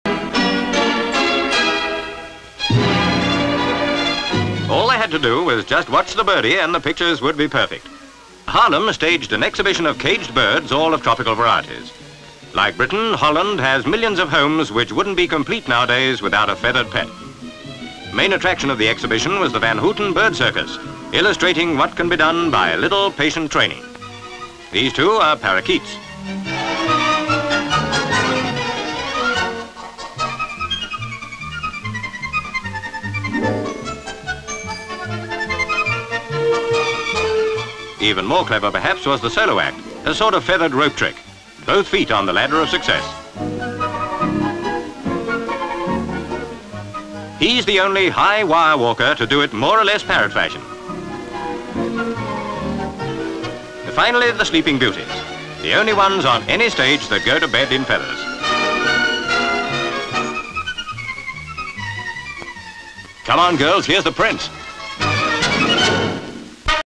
Tropic Birds Exhibition (1958)
Full title reads: "Holland. Tropic Birds Exhibition".

Haarlem, The Netherlands.

SV Numerous cages containing different tropical birds. CU A photographer with a budgerigar on a perch attached to his camera. GV Elevated: the exhibition. CU Man looking at birds in cage. CU A bird in a cage. CU People watching. CU A model motorcycle and sidecar onto which a man puts two birds and the model starts to move round and round. SV The model going round and round with the birds on it watched by people. CU People. CU A bird climbing a miniature rope ladder. CU Man watching. CU The bird walking along a tight-rope. CU Man putting two birds in miniature beds. CU Man putting two birds in miniature beds, he moves his hand away to show the birds laying on their backs with their feet in the air. CU Birds laying on their backs with their feed in the air, they very quickly turn over and get to their feet.

(F.G.)
 FILM ID:1563.24